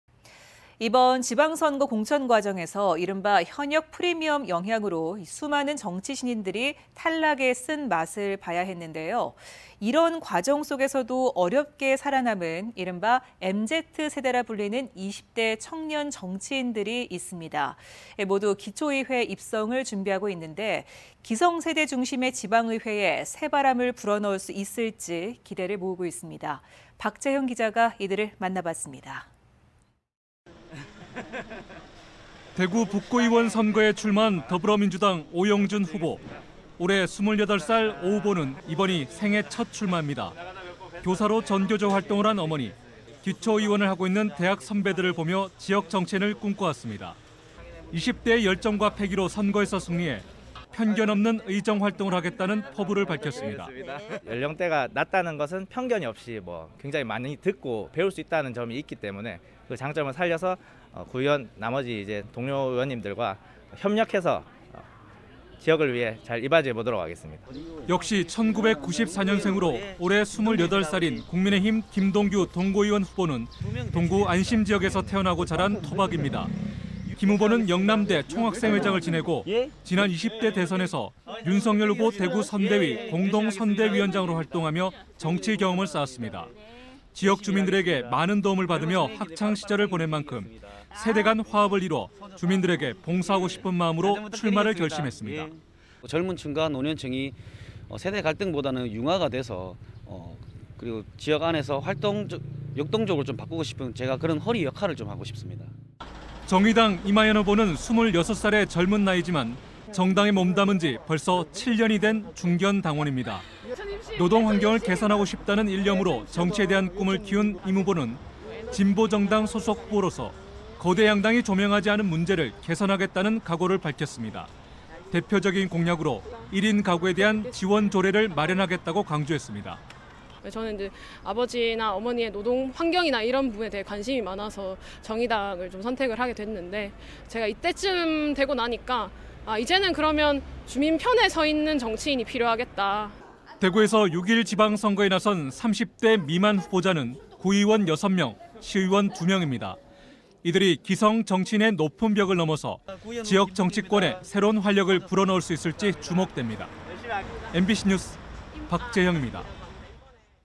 대구 20대 청년, 정치신인들의 지방선거 도전 | 대구MBC뉴스
이번 지방선거 공천 과정에서 이른바 '현역 프리미엄' 영향으로
수많은 정치신인들이  탈락의 쓴 맛을 봐야했는데요.

이런 과정속에서도 어렵게 살아남은
이른바 'MZ 세대'라고 불리는  20대 청년 정치인들이 있습니다.

모두 기초의회 입성을 준비하고 있는데
기성세대 중심의 지방의회에 새바람을 불어넣을수 있을지 기대를 모으고 있습니다.

내용: 대구 20대 청년, 정치신인들의 지방선거 도전 - 박재형 기자